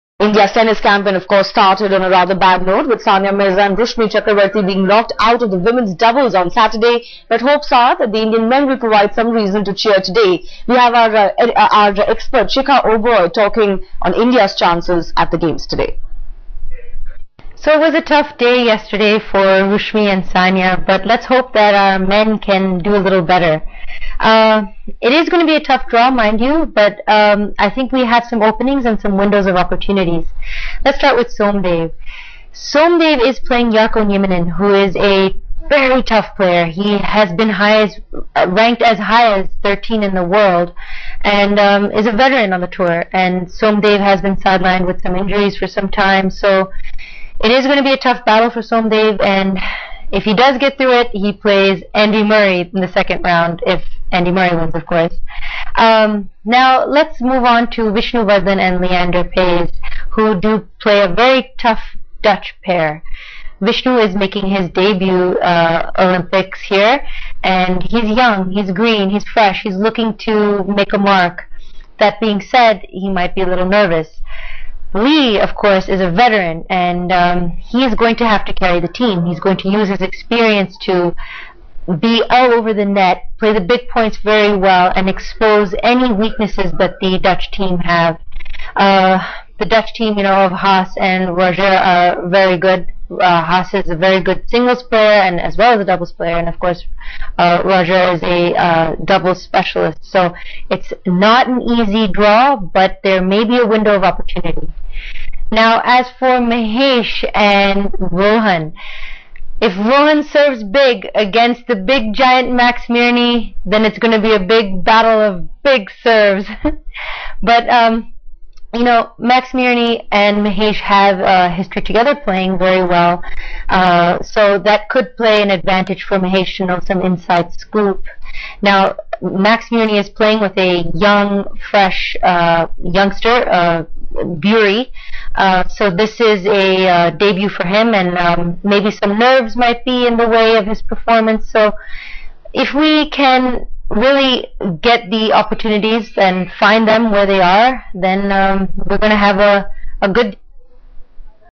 It will be a tough match for Somdev-3
Indian tennis player Somdev Devvarman will play against Jarkko Nieminen from Finland in Men's singles first round today.